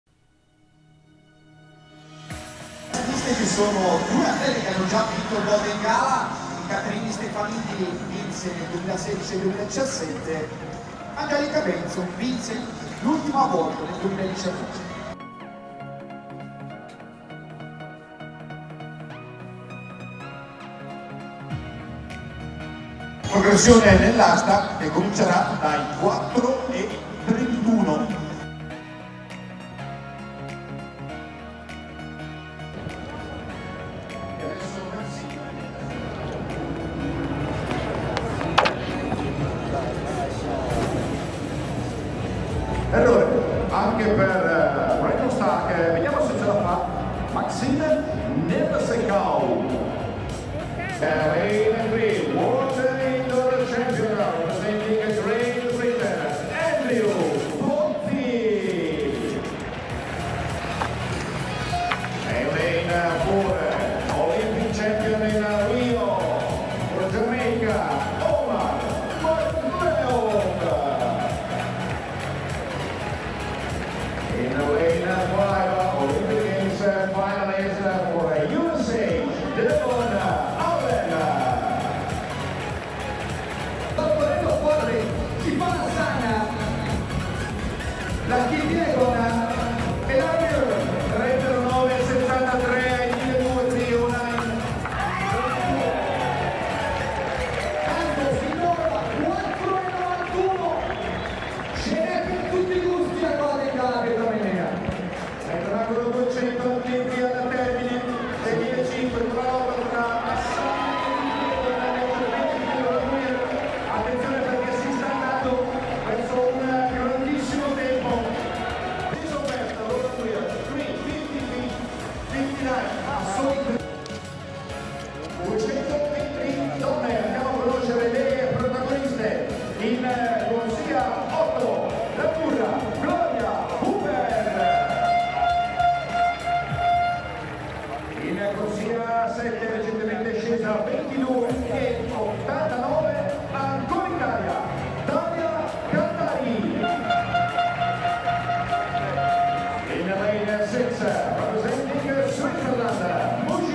GGPM SHORTS 17: Under Pressure (Women's Pole Vault). Golden Gala Pietro Mennea. Firenze, IT. 10/6/21
This video shows the heavy hitters on this competition performing under pressure for the right to take home the points adjudicated to the winner of the Women's Pole Vault Competition at the Golden Gala Pietro Mennea Edition 2021, celebrated at the Luigi Ridolfi Stadium in Florence, Tuscany, Italy on June 10, 2021.

Notable Stars in this video:

Katerina Stefanidi (Greece): 2016 Olympic Champion.

Iryna Zhuk (Belarus): National (Both Outdoor & Indoor) Record Holder.

Anzhelika Sidorova (Authorized Neutral Athlete): Reigning World Champion.

Video Editing, Research, and Digital Art Work By: Carlos G. Monzon-Guzman.